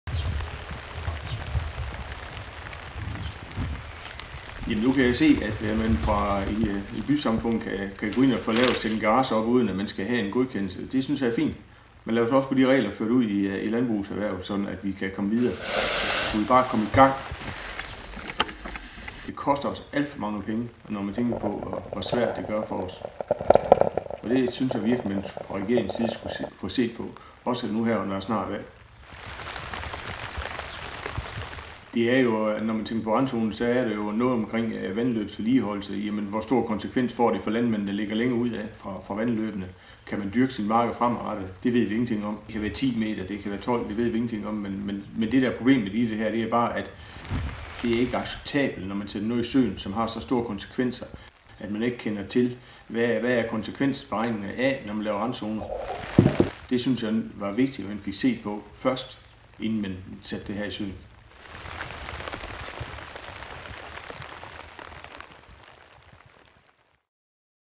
BONDE-BLOGGEN JUNI 2011
Det koster landbruget kassen, at regeringen ikke laver lige så lempelige vilkår for landbrugserhvervet som for byfolket.

Og det er ikke acceptabelt at sætte kravet om randzoner i søen - når ingen kender konsekvenserne af loven, siger formanden for Vestjysk Landboforening, Søren Christensen på Bonde-Bloggen i juni.